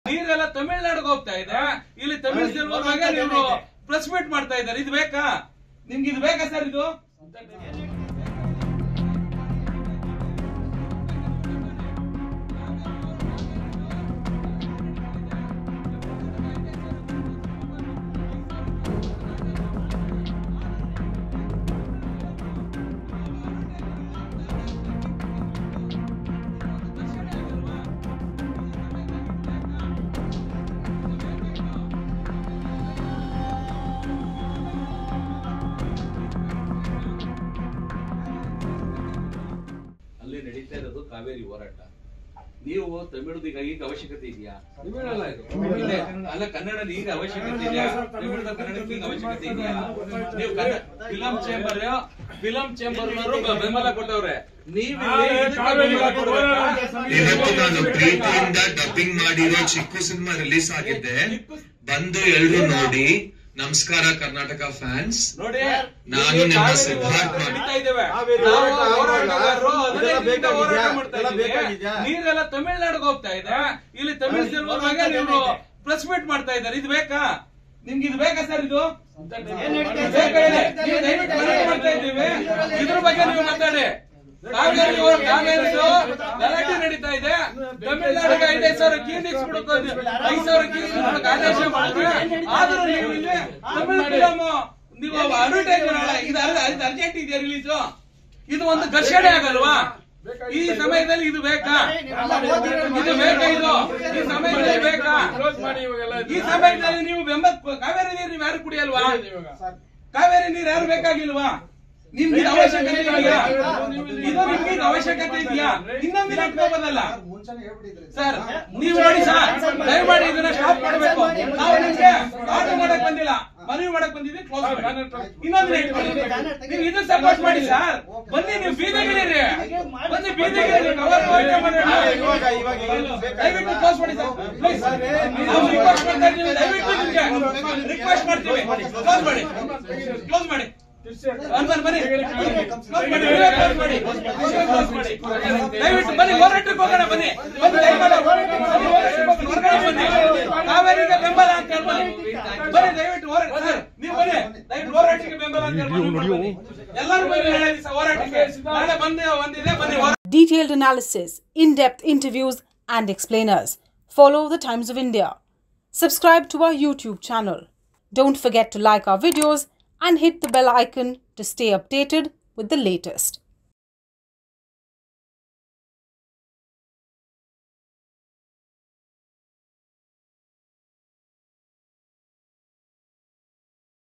Actor Siddharth's press meet for promotion of Tamil film ‘Chikku’ disrupted by pro-Kannada activists
Members of Karnataka Rakshana Vedike Swabhimani Sene disrupted the press meet of actor Siddharth, who was promoting his Tamil film Chikku in Bengaluru. Protesters say this was not the right time to promote a Tamil film in Bengaluru. Protesters also asked the actor to express support for Karnataka in the Cauvery dispute case. Siddharth stopped his press meet and then left the venue after some time.